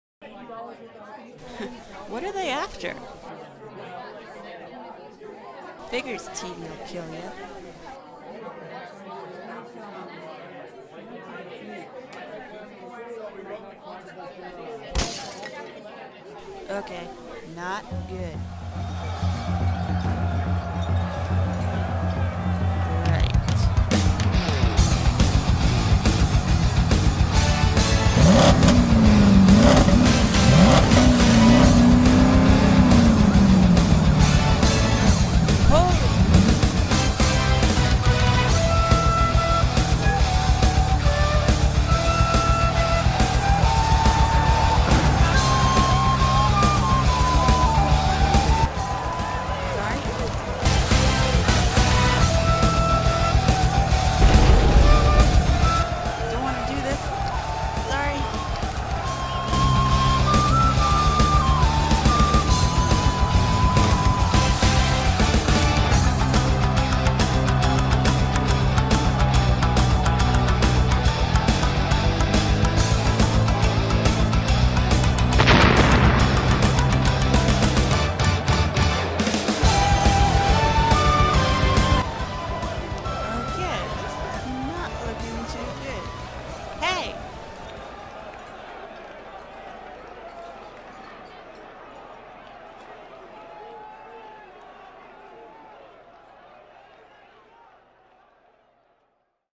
Mob Fight (Storyboard with Sound)
This is a storyboard done in 2009 with music , effects redone.